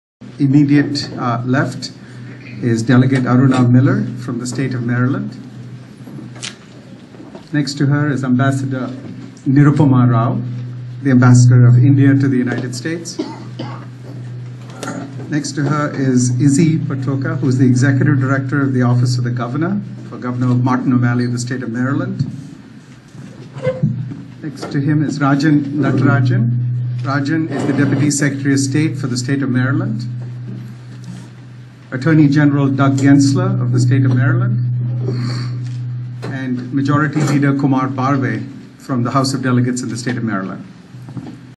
Introduction of attending Ieaders at the Sri Siva Vishnu Temple in Lanham, Maryland
Introduction of attending Ieaders from a wide spectrum of faiths and political parties joined the top Indian diplomat Nirupama Rao from Washington, DC, at the Sri Siva Vishnu Temple in Lanham, Maryland to emphasize the need to engage with one another, promote awareness, and eradicate ignorance in hopes of preventing such acts of hatred in the future.